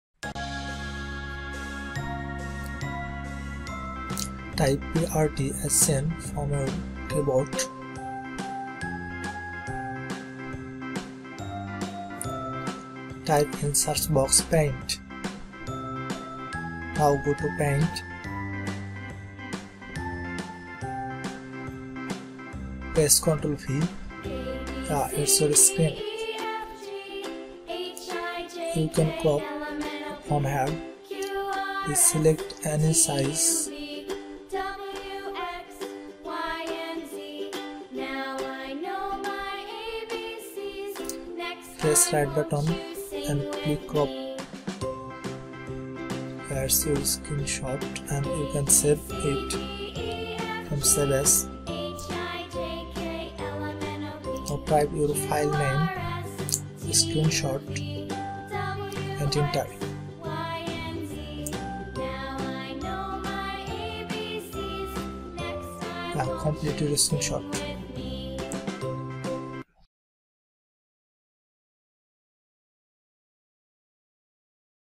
How to take screenshot by computer | Screenshot by print screen on PC | Laptop screenshot by paint
This is very simple process. First press the print screen or prt scn button on key board . Then open pain from windows accessories and paste. And follow the video.

All computer/ laptop/ mobile/ graphics tips and tricks.
How to download android apps by pc easy way.